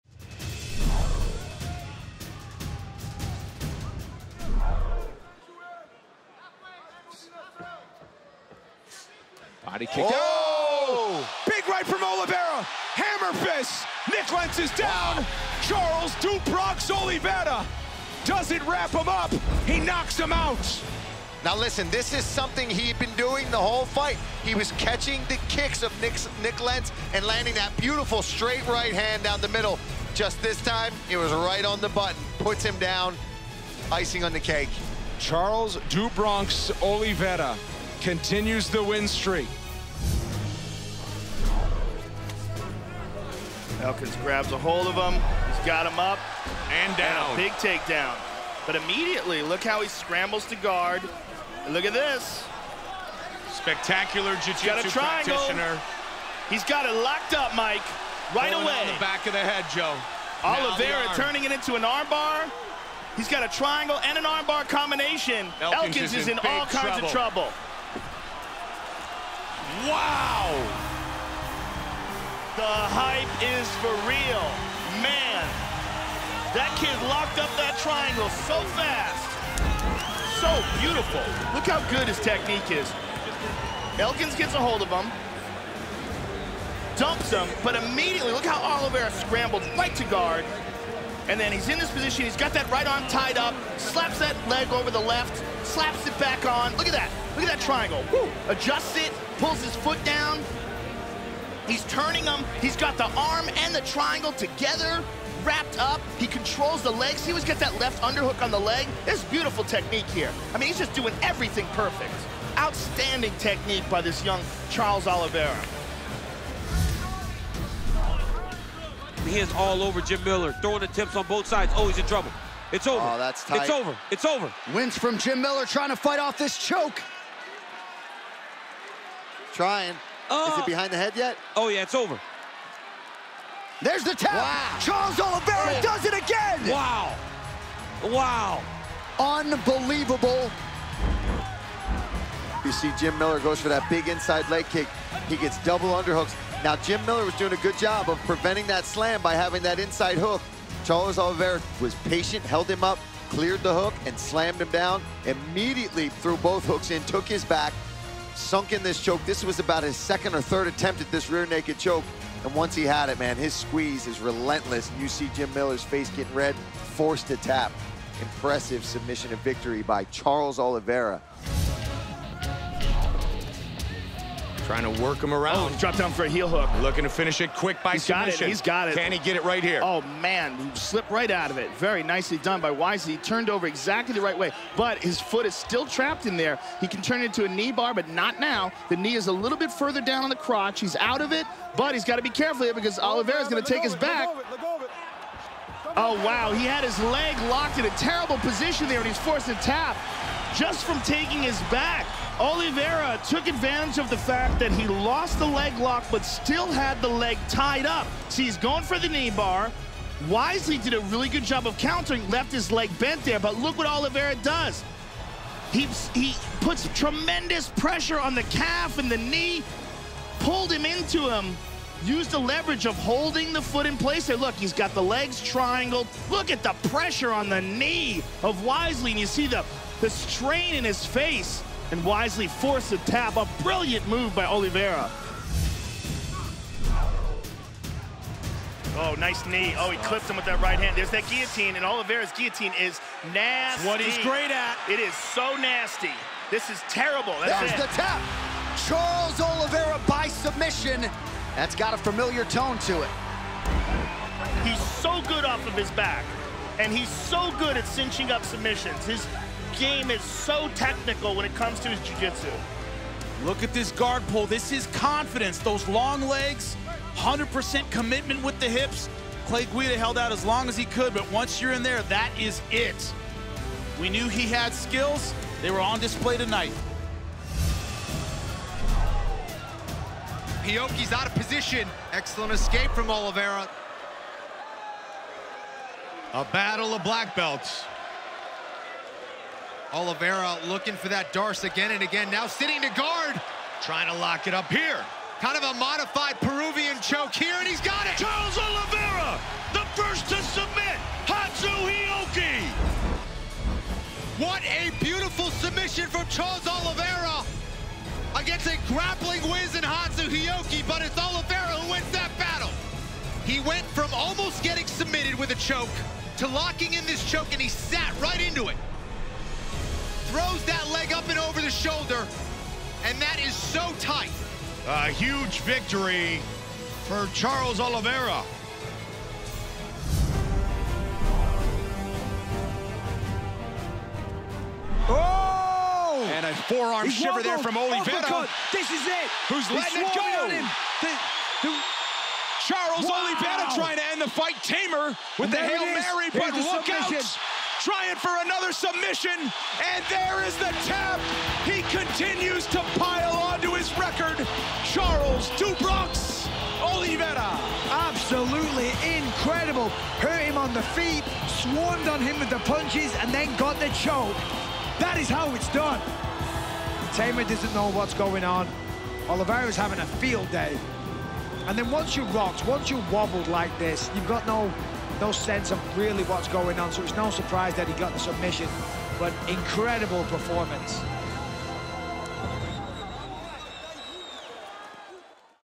Top Finishes: Charles Oliveira
Charles Oliveira holds the UFC record for most submission victories with 13 to date. Take a look back through his top finishes in his career.

Experience UFC live with UFC FIGHT PASS, the digital subscription service of the UFC.